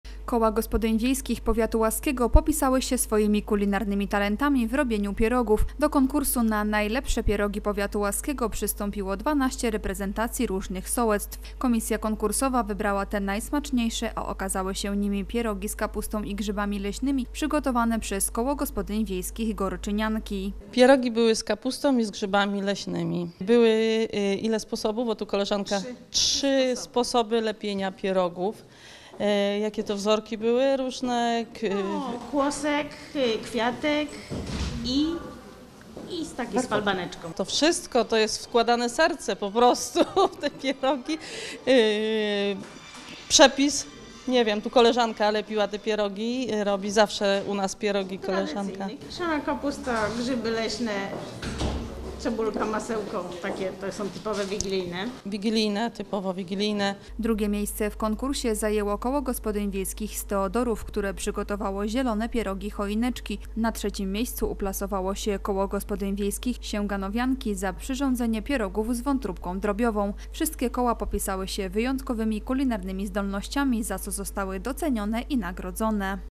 Konkurs pierogowy w Powiecie Łaskim
Tradycyjne, wigilijne pierogi z kapustą i grzybami, a może z farszem z kaczki, gęsiną lub wątróbką drobiową? Ciekawe, które Państwu posmakowałyby najbardziej. Takie właśnie rarytasy z powiatu łaskiego stanęły do rywalizacji.